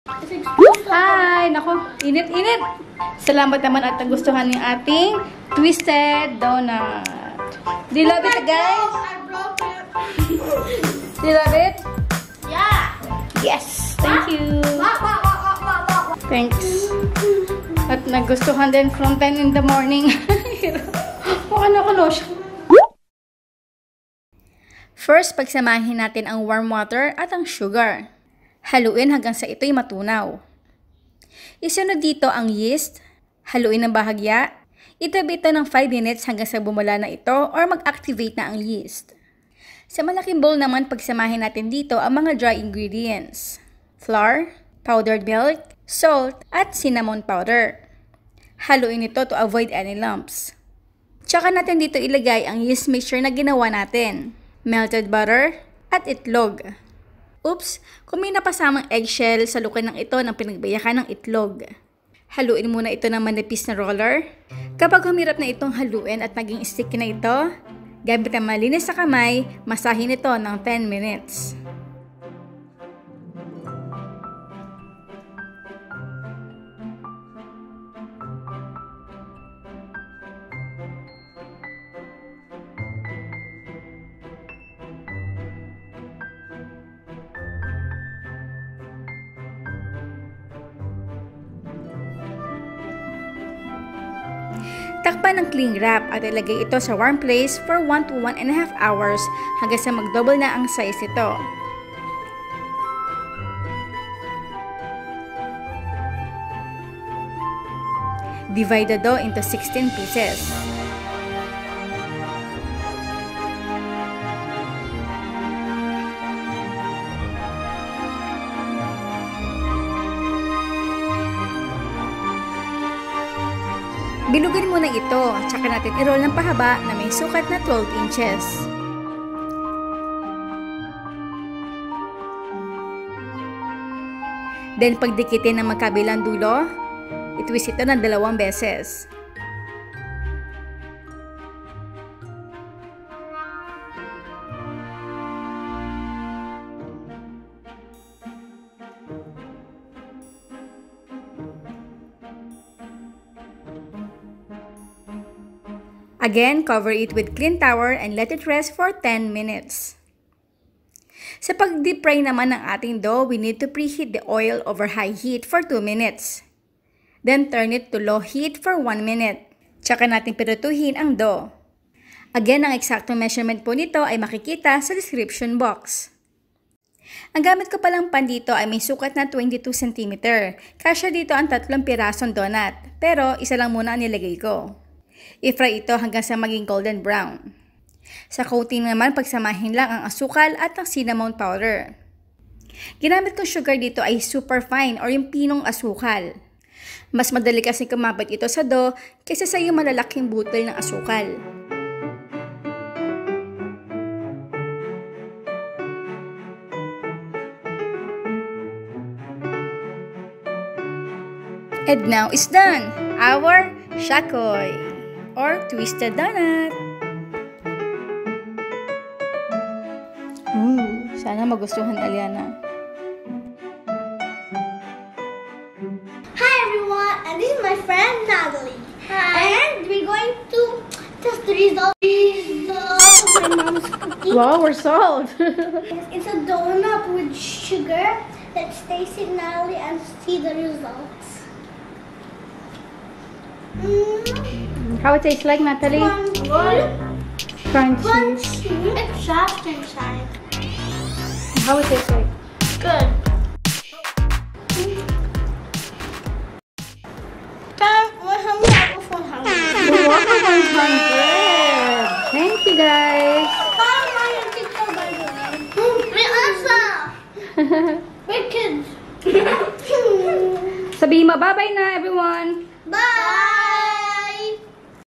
SHAKOY ( Twister Donut) ganito ang gawin mo! below 100 Pesos Patok pang negosyo | ANG SARAP GRABE
SHAKOY | BICHO-BICHO | TWISTED DONUTS

2 cups na HARINA, ganito ang gawin mo! below 100 Pesos Patok pang negosyo at mapapa ANG SARAP GRABE

fluffy and soft shakoy | bicho-bicho or twisted donut, ayan ang masasabi ng dalawang bata sa video.. muntik na nlia maubos ang ginawa kong shakoy buti nalang tinago ko na ang garapon :)

Ingredients
1/2 cup plus 2 tablespoon warm water
2 teaspoon yeast
2 tablespoon sugar

2 cups flour
1/4 cup powdered milk
1 teaspoon salt
1/2 teaspoon cinnamon powder

1 egg
3 tablespoon melted butter

after raising the dough cut into 16 pieces or 34-36 grams each
each dough, roll to ball then to a log with 12 inches length
raise again for 10 minutes (covered with clean towel)

for frying 2 cups oil
I used 22 centimeter pan
fry in preheated (napainit ng oil) over low heat
fry 3 pieces at a time until golden brown

for coating mix
1/3 cup super fine (pinong pino) sugar
1/2 teaspoon cinnamon powder

Yield : 16 pieces

POST ON FACEBOOK ON APRIL 2, 2021